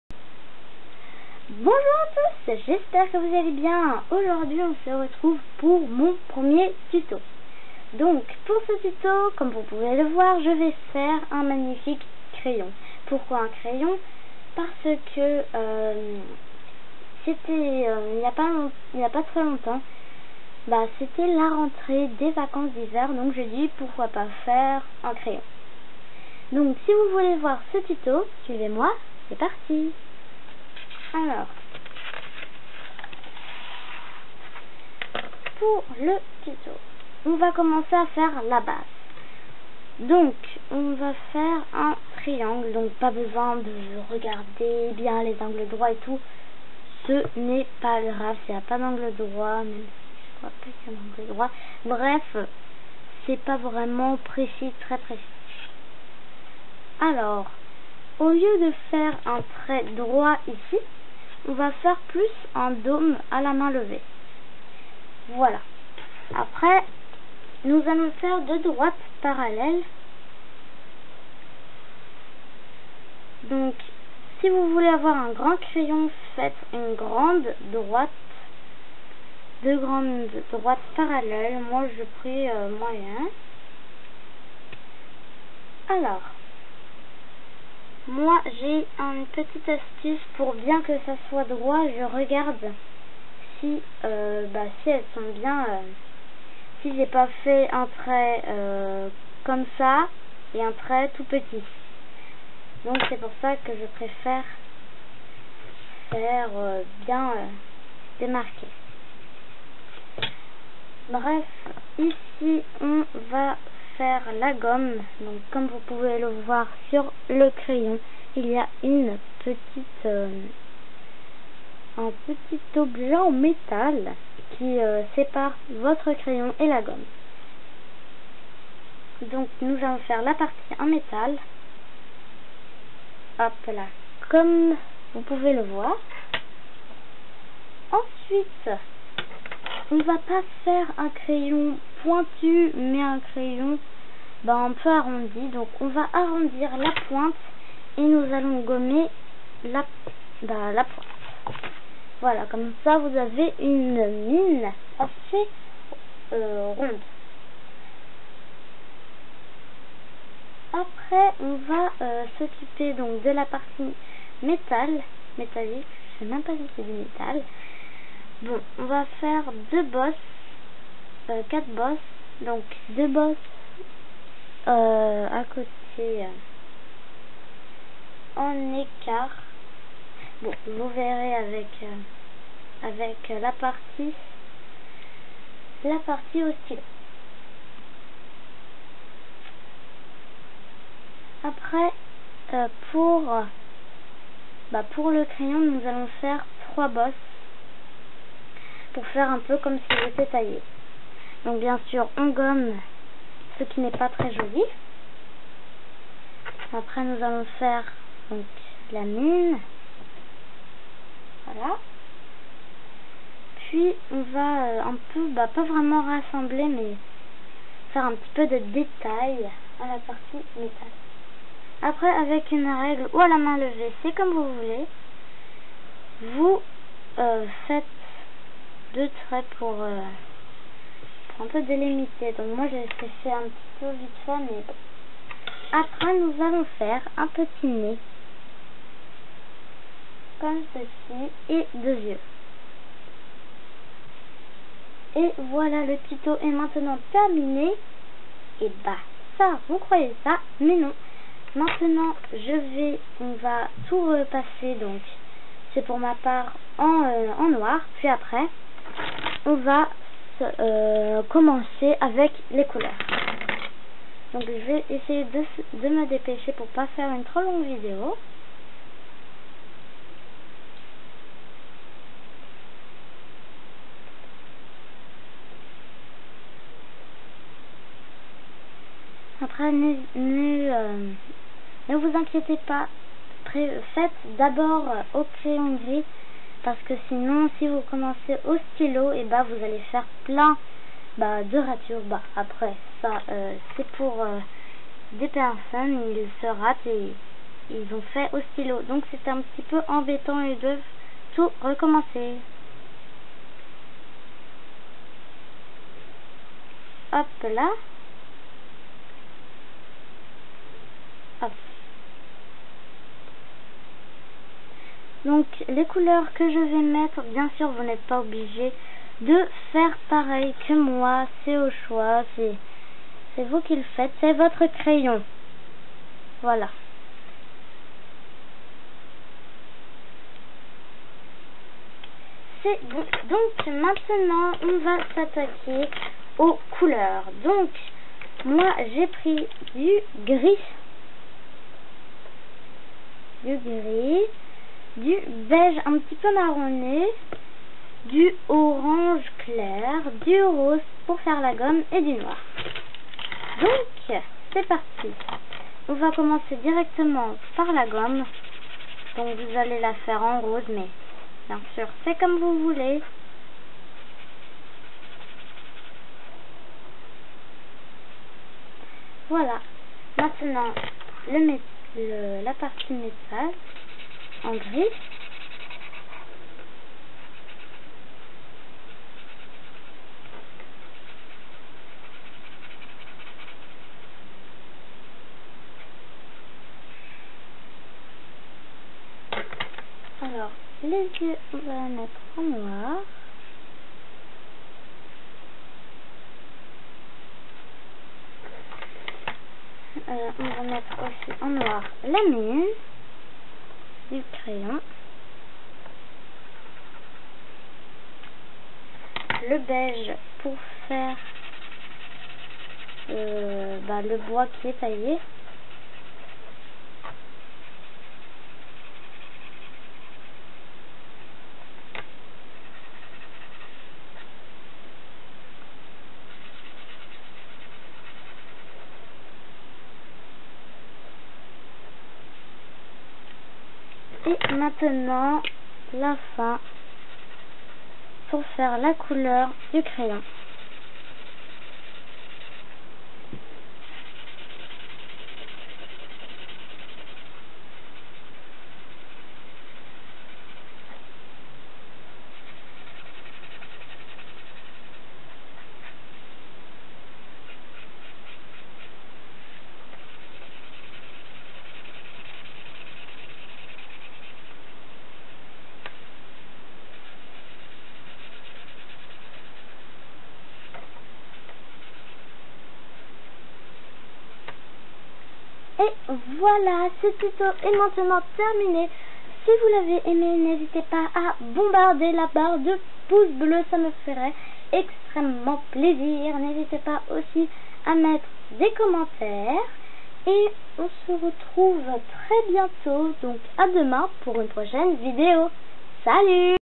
Tuto Dessin crayon - La Cabane d'Eva
Je vous présente mon premier tuto dessin. Il s'agit de dessiner un crayon.